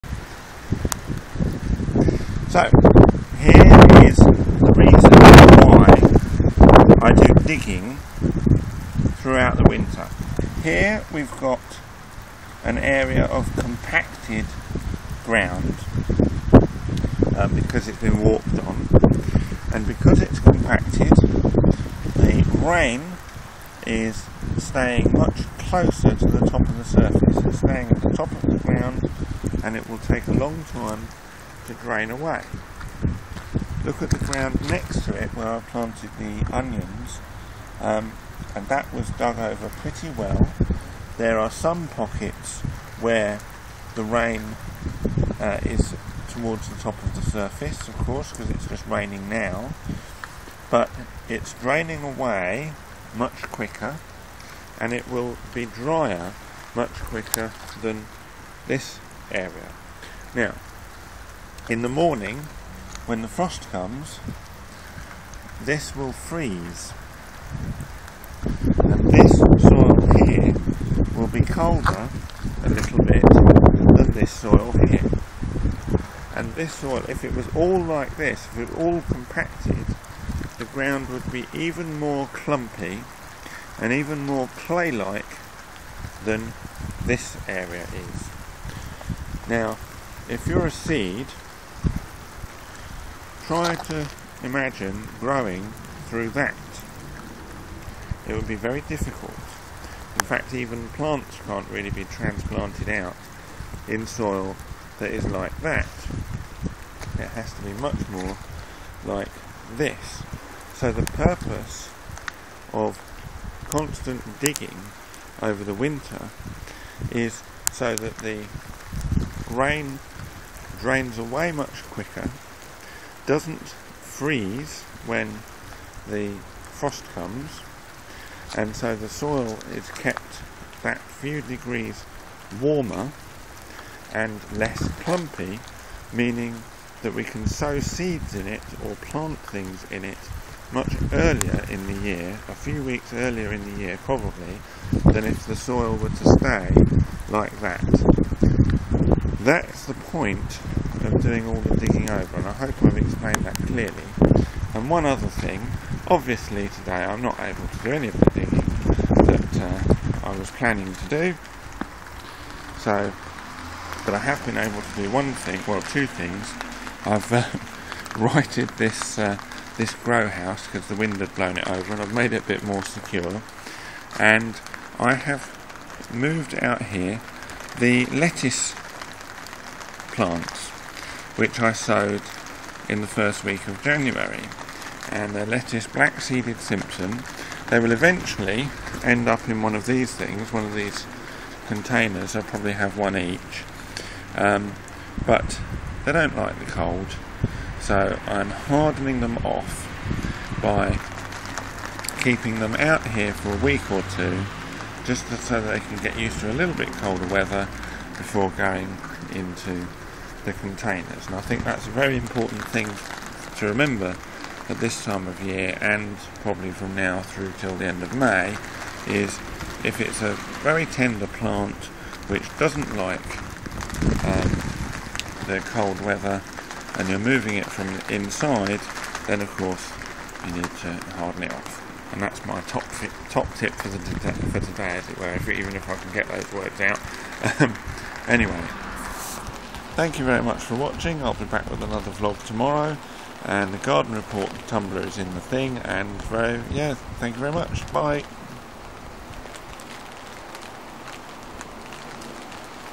Allotment Garden Report: Soil Talk & Hardening-off Lettuces
An update on what's going on in the garden.